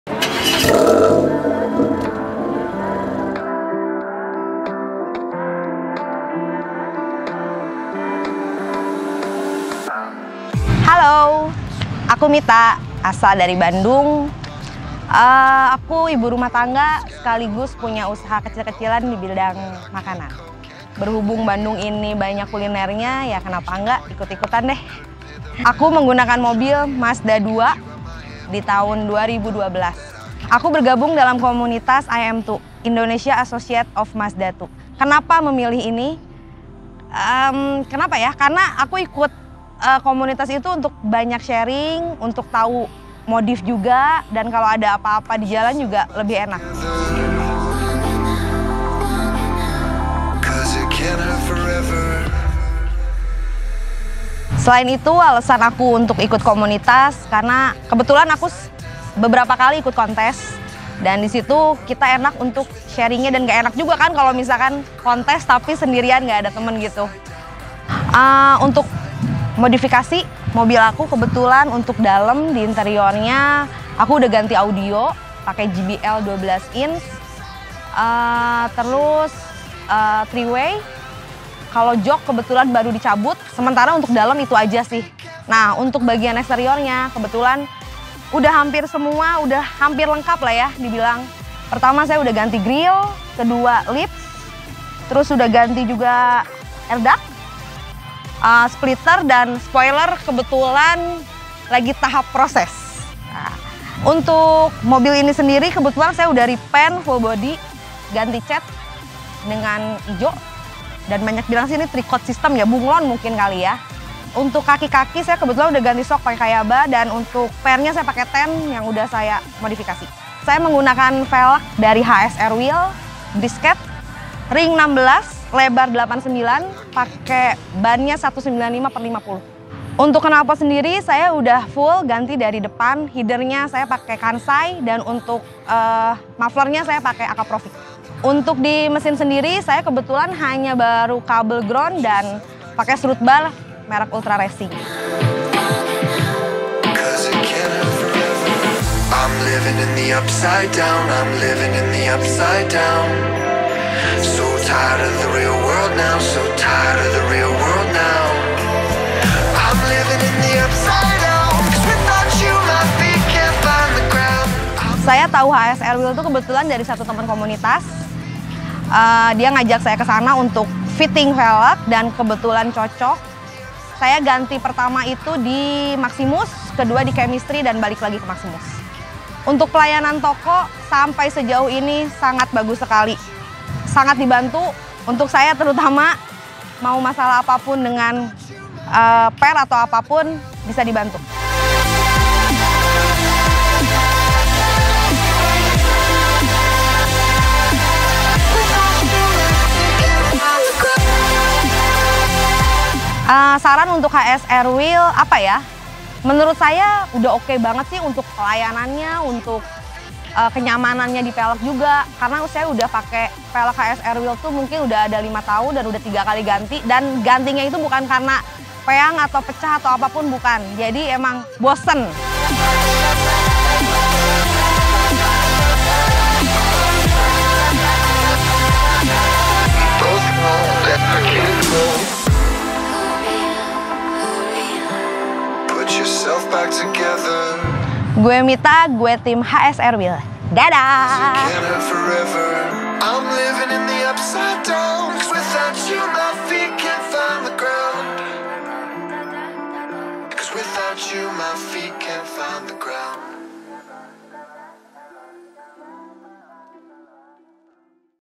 Owner's Spotlight - Modifikasi Mazda 2 Pakai Velg HSR BRISKET Untuk Kontes
TEAMHSR HUNTING CAR. teamhsr lagi cari unit mobil nih untuk shooting. Buat Haiser yang mau berbaik hati minjemin mobilnya untuk shooting, klik link whatsapp ini ya . Terima Kasih Haiser.
Di Owner’s Spotlight kali ini, teamhsr mendatangi Haiser Bandung yang sudah modifikasi Mazda2 miliknya menggunakan velg HSRWheel.
Tertarik dengan velg di video ini? Klik link di bawah.